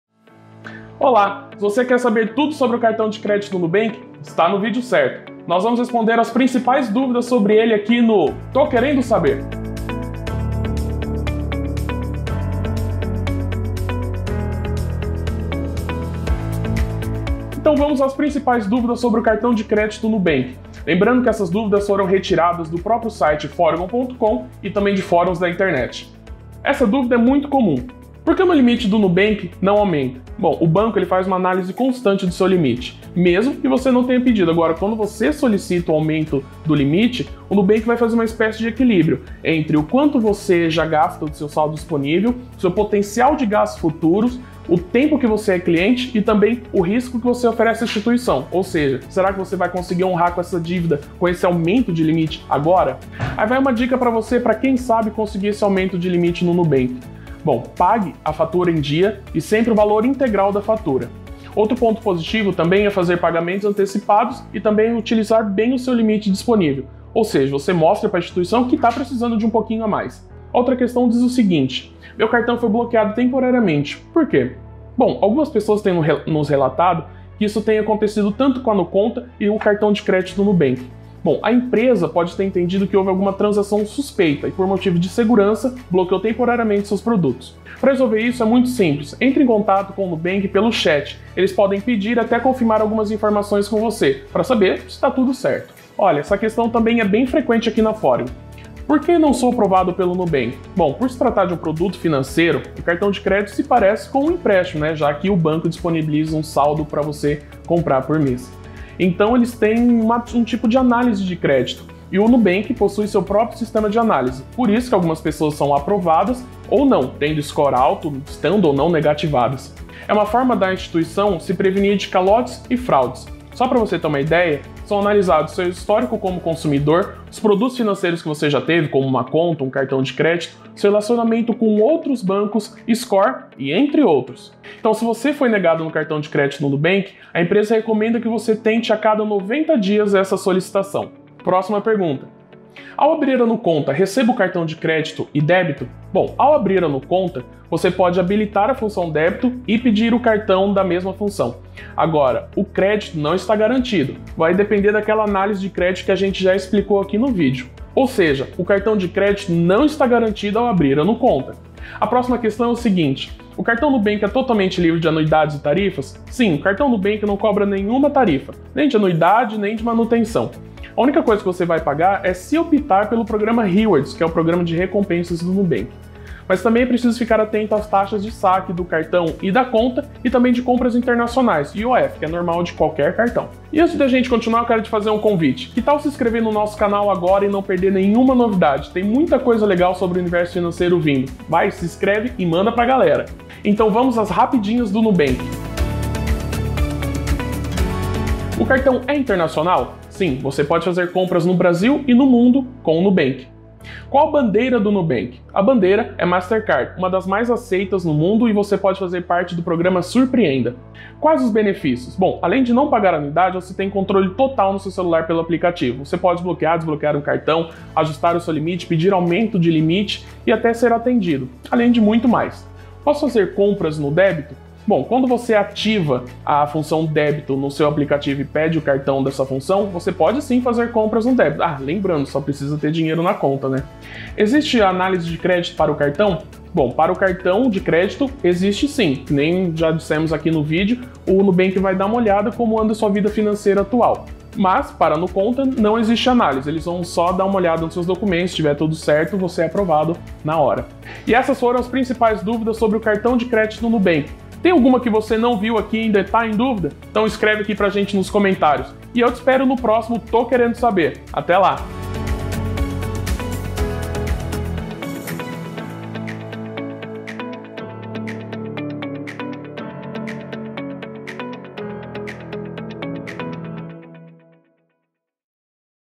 Perguntas FREQUENTES sobre o Cartão de Crédito NUBANK
O Banco Digital nubank é o queridinho de muita gente 💜 Mas você sabe como funciona o seu Cartão de Crédito? Nesse vídeos nós vamos sanar as principais dúvidas sobre ele no quadro "Tô querendo saber"!

Confira quais são as principais dúvidas: 👇

❓ Por que o meu limite do Nubank não aumenta?
❓ Meu cartão foi bloqueado temporariamente, por quê?
❓ Por que não sou aprovado pelo Nubank?
❓ Ao abrir a Nuconta recebo cartão de crédito e débito?
❓ O cartão Nubank é totalmente livre de de anuidades e tarifas?
❓ O cartão é Internacional?
❓ Qual a bandeira do Nubank?
❓ Quais os benefícios?
❓ Posso fazer compras no débito?
❓ Existe análise de crédito para o cartão?

Confira todas as respostas para essas perguntas no vídeo! 👆

Você tem alguma dúvida que não apareceu por aqui? Escreva para nós aqui nos comentários 😉

Te esperamos no próximo "Tô querendo saber", até lá!